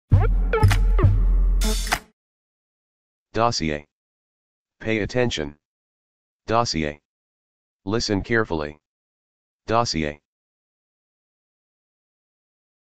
Dossier Pronunciation ⚡️ How To Pronounce Dossier!
🆗How to pronounce dossier  in this video!

Do you want to improve your English pronunciation? Well, you've come to the right place. In this particular video you can watch how to pronounce dossier precisely.

You can improve your English accent with these videos and sound like a native.

👉Listen to how to pronounce dossier at this time!